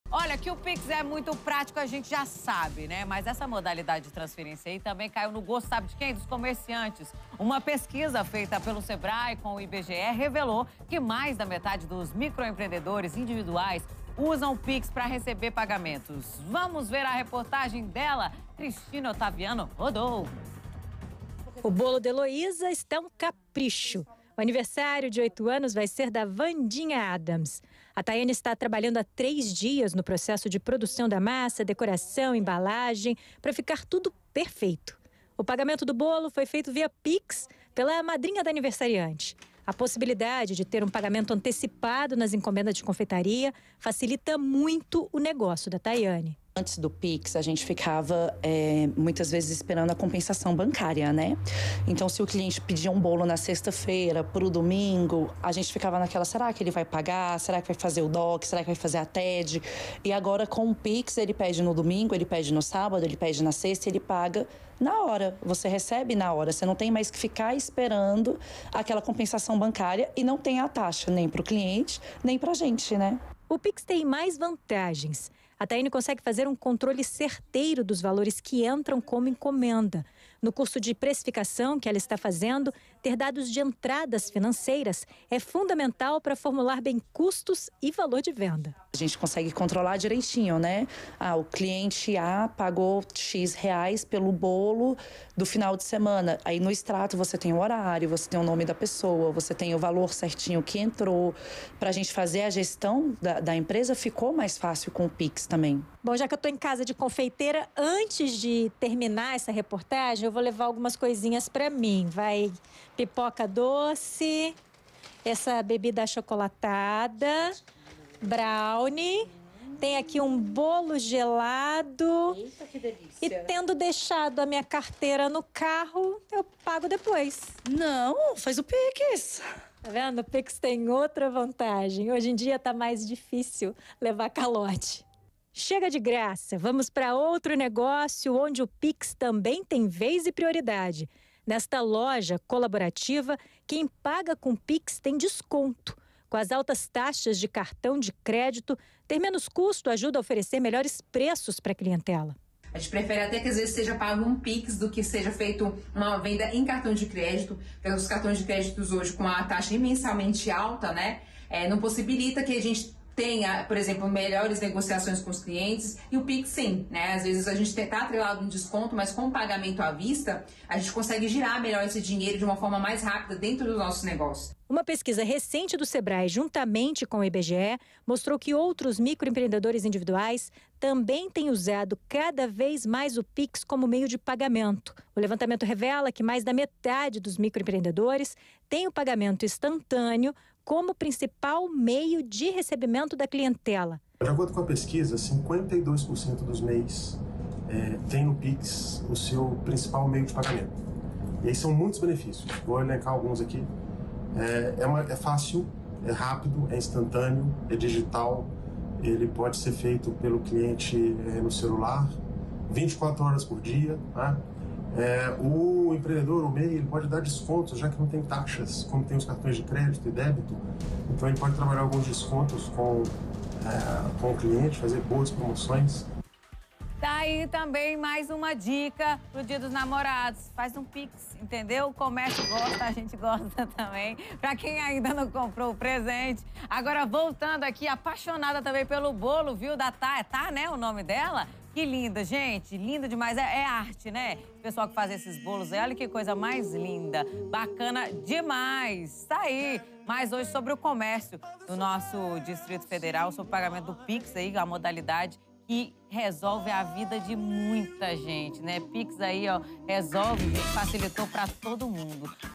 Pix é a principal modalidade de pagamento usada entre os empreendedores, diz estudo
O Pix é a modalidade de pagamento mais usada entre os microempreendedores individuais, aponta uma pesquisa feita pelo IBGE em parceria com o Sebrae. Entre os 7.537 empreendedores que participaram do levantamento, 52% consideraram a modalidade como o principal meio para receber pagamentos.